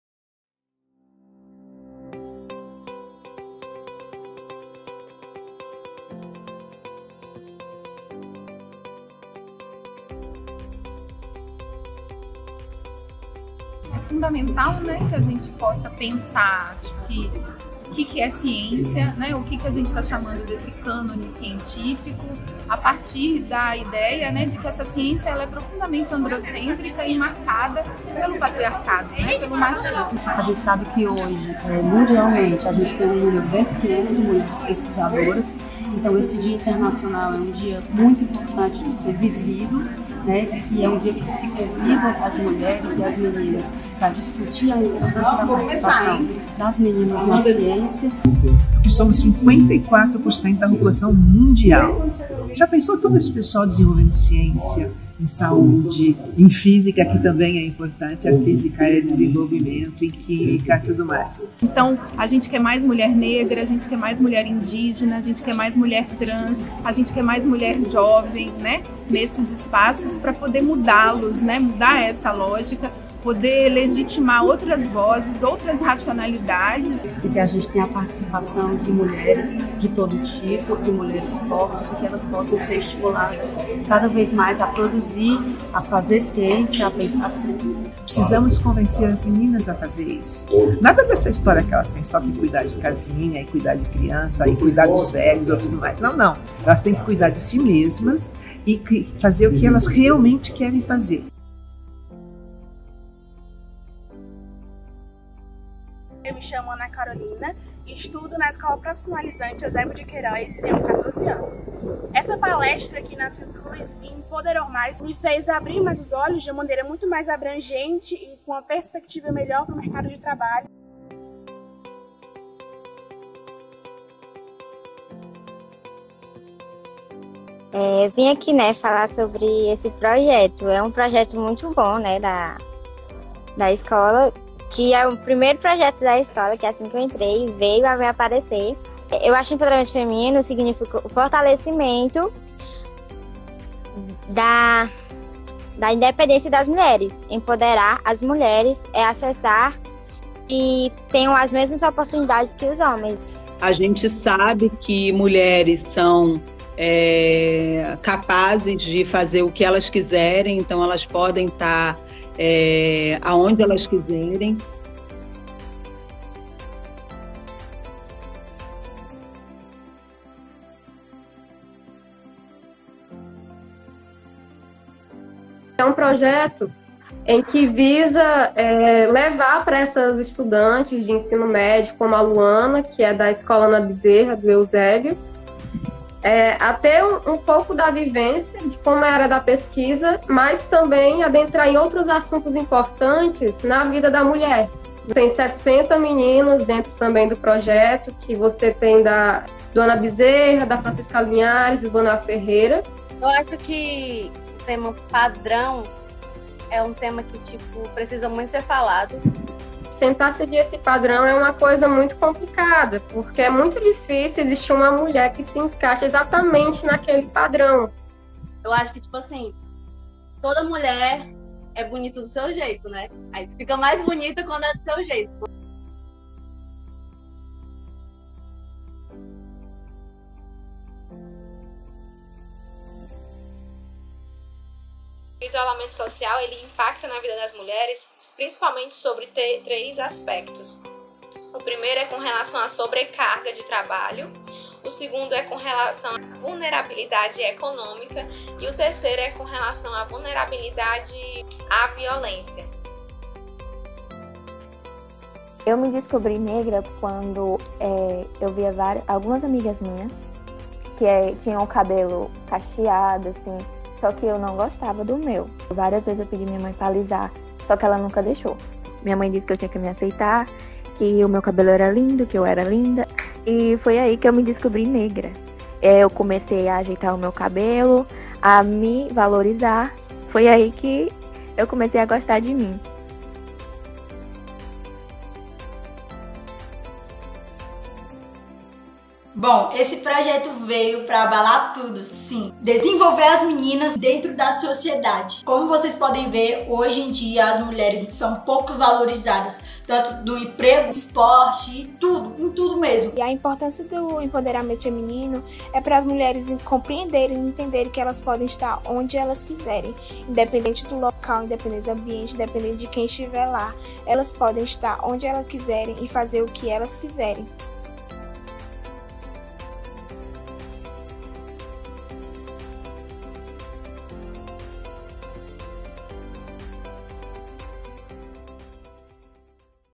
Dia Internacional das Mulheres e Meninas na Ciência - Fiocruz Ceará
No dia 11 de fevereiro é comemorado o Dia Internacional das Mulheres e Meninas na Ciência. Nesse vídeo, mulheres e meninas falam sobre a importância de suas presenças nesse espaço de pesquisa.